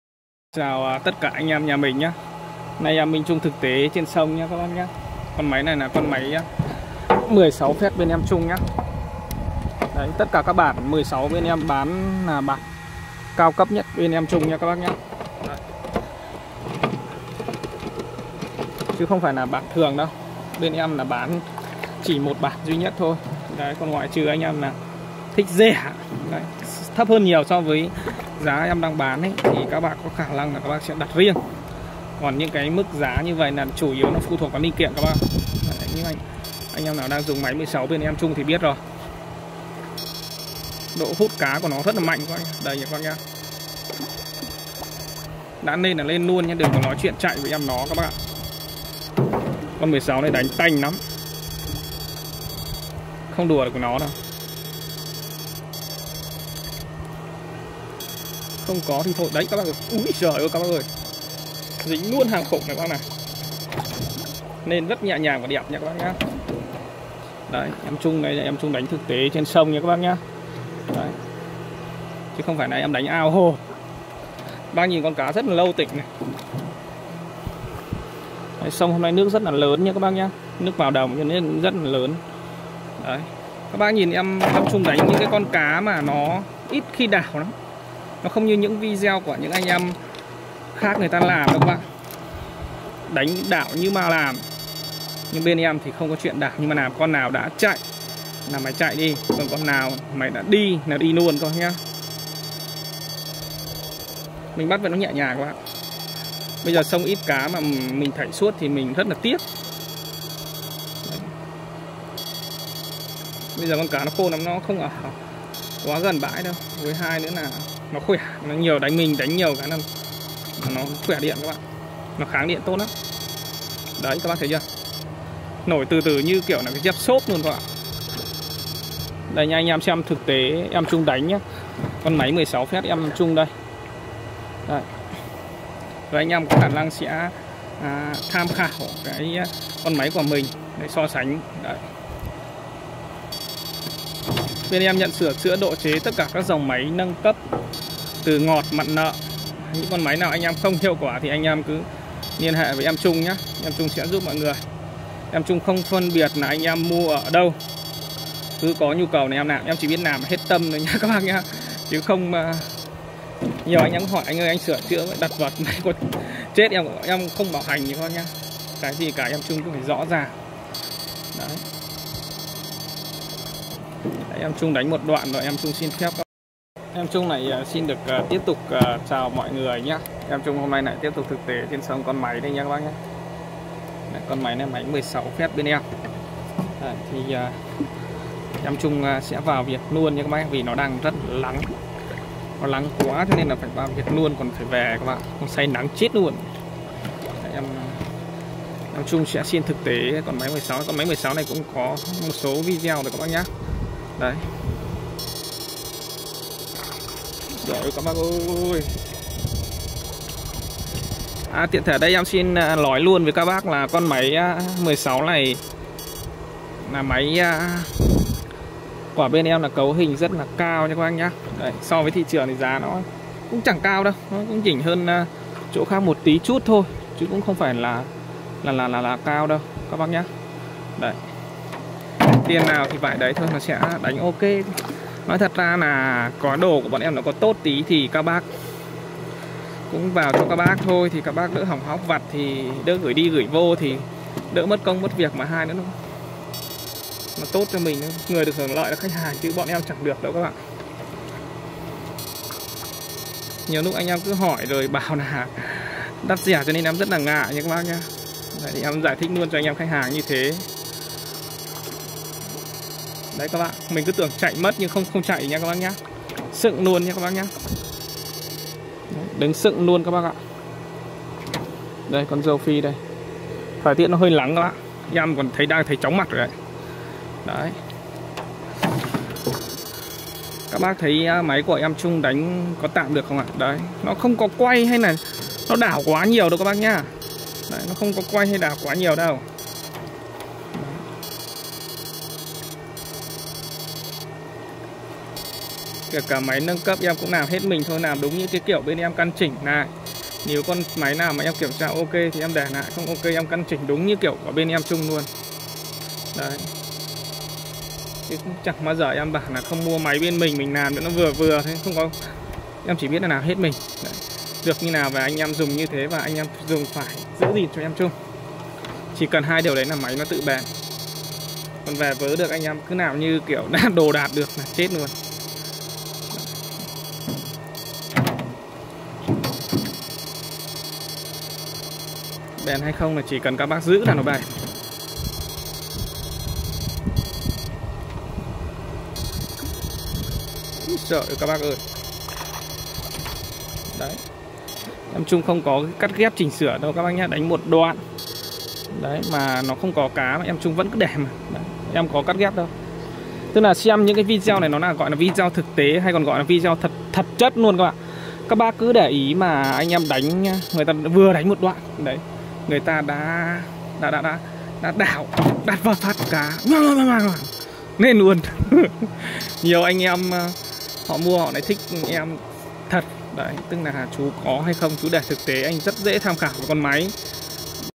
thực tế tet máy 16 fet siêu rô phi v tổng hợp các loại cá trước gửi khách. lh ĐT Zalo 0362921480
thực tế trên sông kích cá điện tử 16 phét trước khi gửi cho ae khách hàng